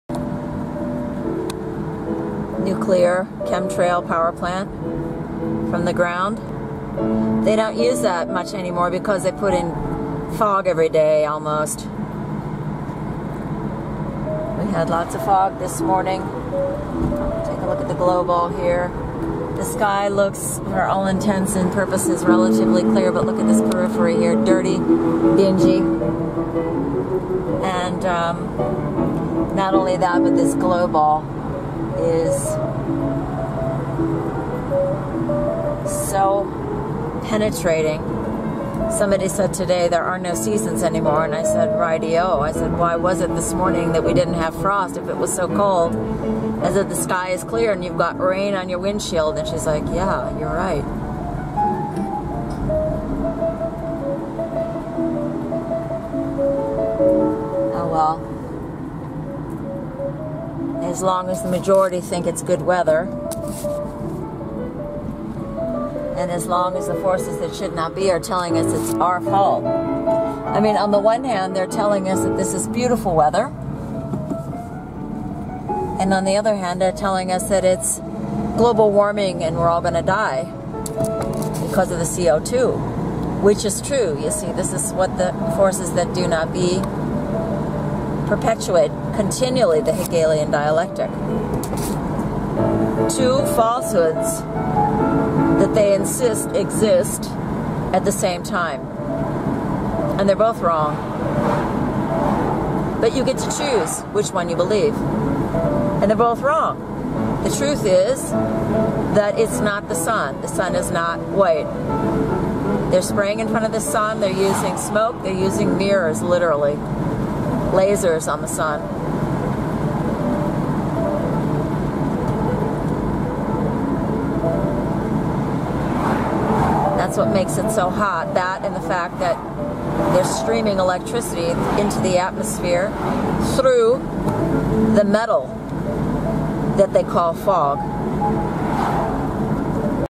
Communistic Meltingpot
Degrading God´s Creation: everything is the same, no differentiation. No more seasons, no more male and female - lines being blurred, people becoming disturbed.